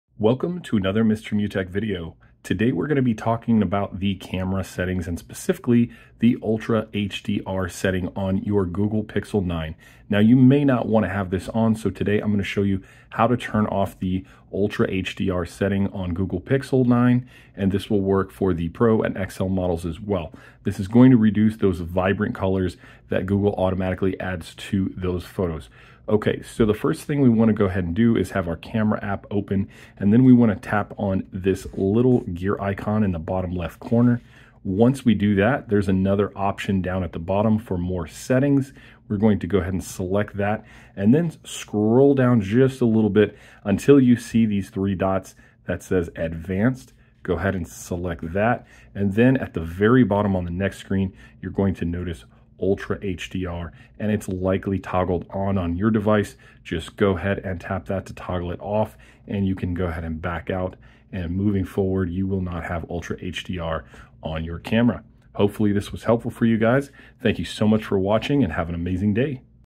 How To Turn Off Ultra HDR On Google Pixel 9
This tutorial shows you how to turn off and disable Ultra HDR on Google Pixel 9 (Pro & XL) cameras. This feature adds additional light and color to photos, and some prefer to have a more natural look without additional filters added.